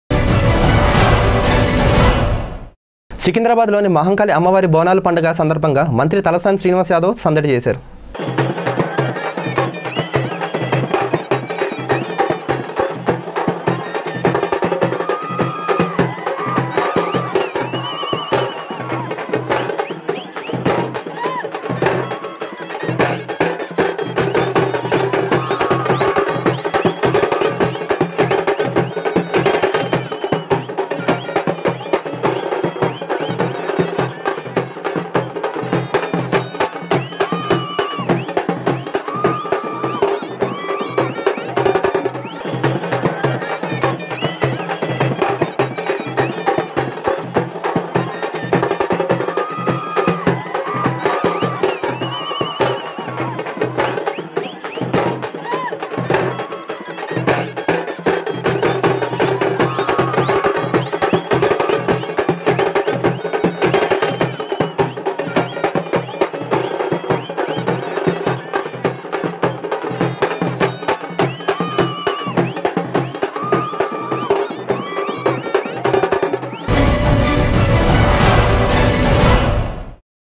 ల‌ష్క‌ర్ బోనాల్లో మంత్రి త‌ల‌సాని తీన్ మార్ డాన్స్ చూస్తే నవ్వు ఆపుకోలేరు || V5 News
Telangana Minister Talasani srinivas yadav Theenmar Dance at Hyderabad Bolala Fesiwal.